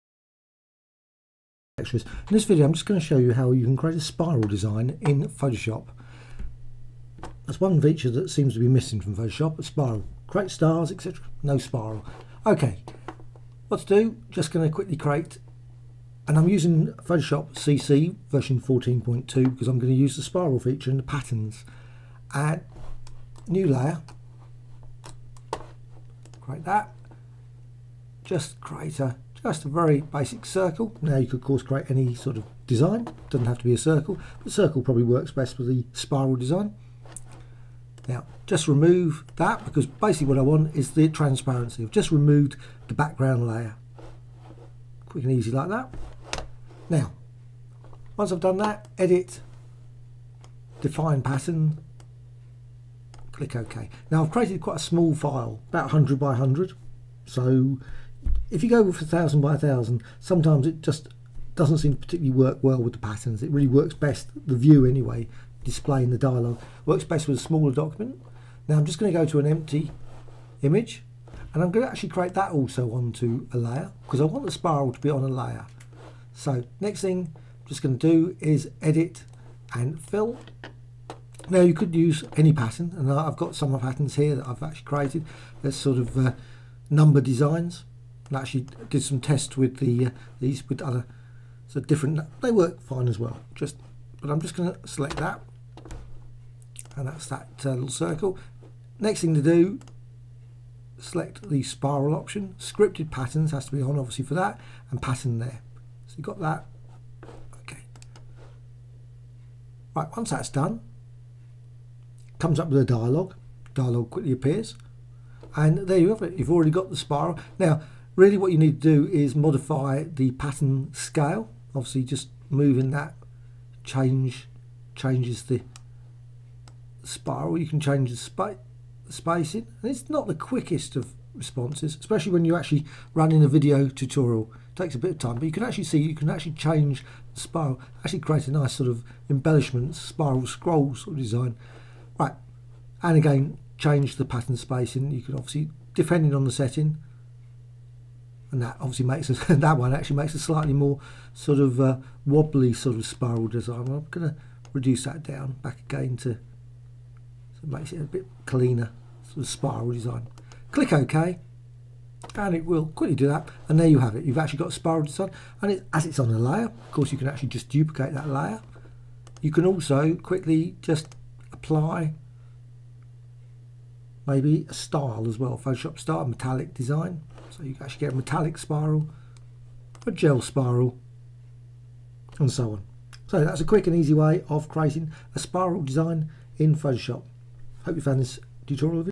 Create SPIRALS In Photoshop
How-to create spiral designs in Photoshop CC  software - create a very basic spiral design. Please note that the spiral does require a little trial and error with the settings and size of shape otherwise you will end up with spaces in your spiral